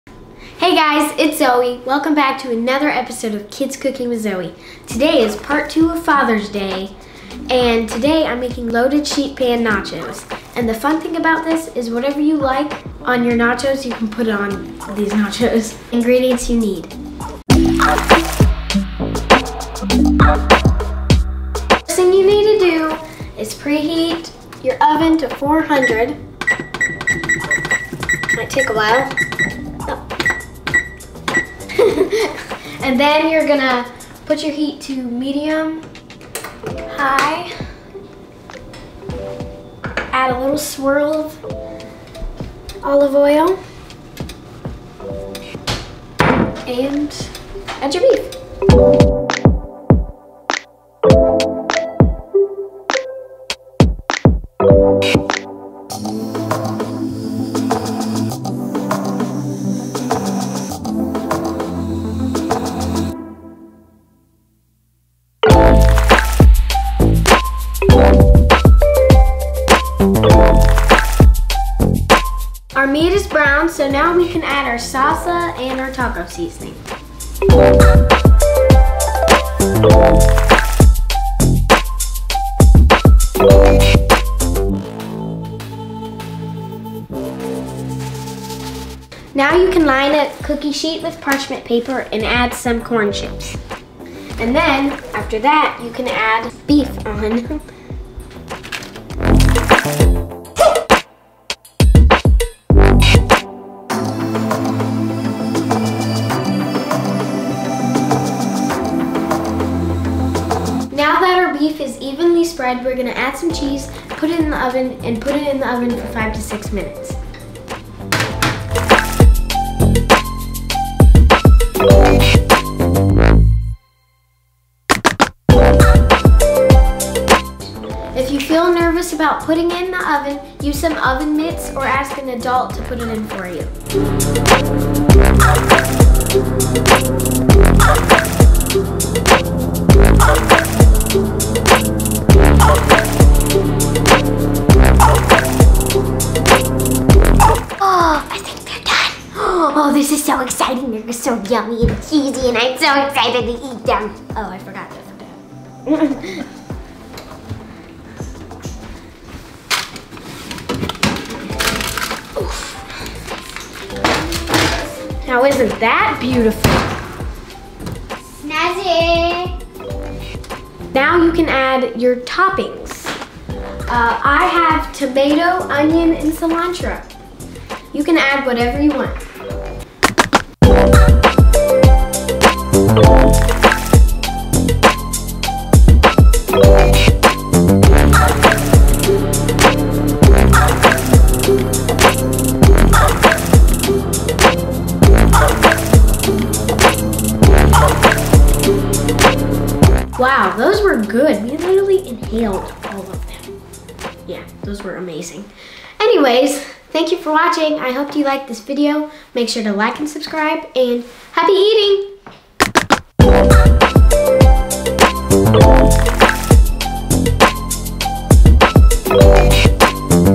Nachos / Father's Day Recipes / Kids Cooking Show
Today on Kids Cooking with Zoe I'm so excited to make Loaded Sheet Pan Nachos for my Dad!!  In just a few days it's Fathers Day and this is one of my Dad's favorites!! Click SHOW MORE below to see the recipe!

Here's what you'll need to make your Dad or loved one Loaded Sheet Pan Nachos:

1/2 lb ground beef
Taco Seasoning
1 cup salsa
Tortilla chips
2 cups Mexican cheese blend
1 Roma tomato diced
Fresh cilantro

You'll cook the meat, add them to the top of the nachos, then add the cheese and bake at 400 for 4 minutes and add the rest of the toppings! Easy peasy! Happy eating!!